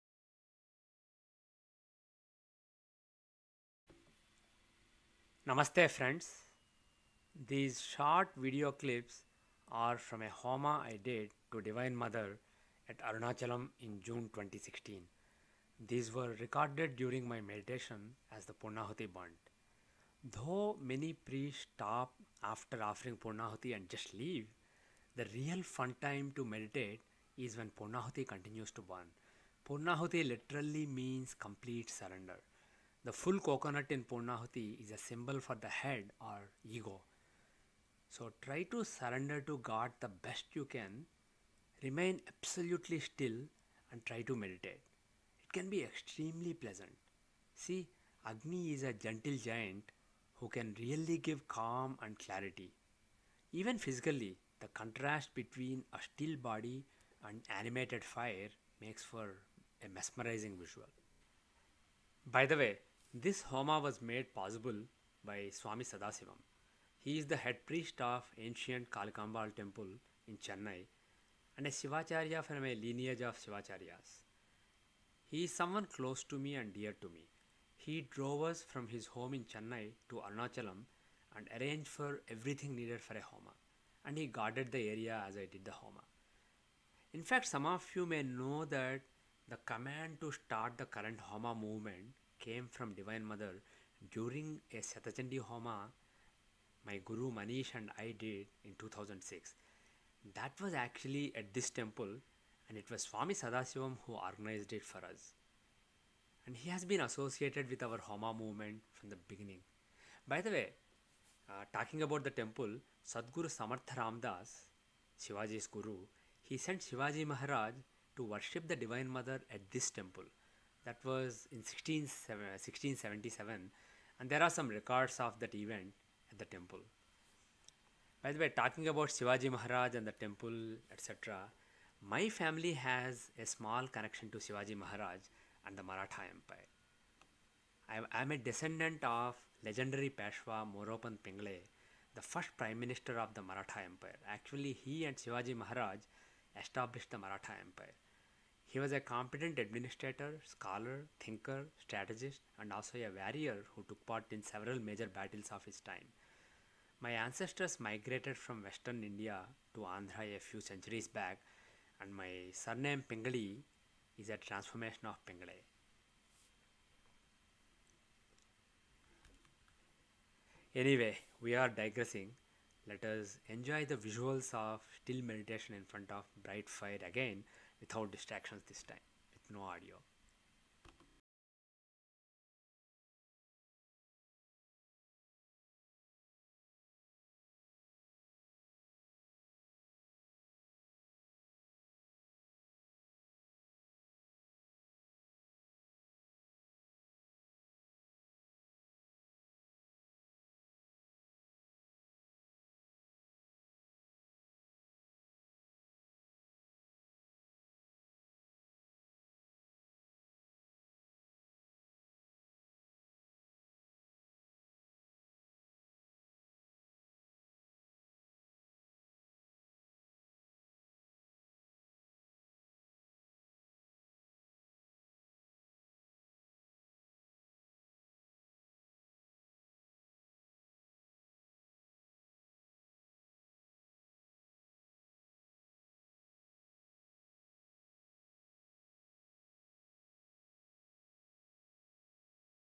Still Meditation with Homa (and some tidbits & connections)
In this short 4-min video, you can enjoy the visuals of my still meditation in front of animated homa fire, while I speak in the background and share little tidbits and some connections.

Agni is a gentle giant who can give calm and clarity. Surrendering to God, sitting still and meditating in front of bright and animated fire is very pleasant! 🙏🔥🕉🔥🙏